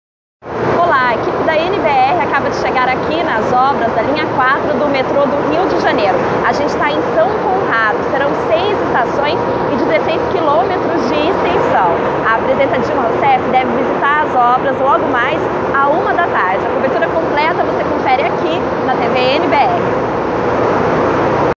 Equipe da TV NBR visita obras do metrô do Rio de Janeiro
ESPECIAL WEB - 12.05.15: A presidenta Dilma Rousseff visita nesta terça-feira (12) as obras da linha 4 do metrô do Rio de Janeiro, que terá 16 km de extensão em 6 estações. A equipe da TV NBR já está na Estação São Conrado e acompanha a visita da presidenta.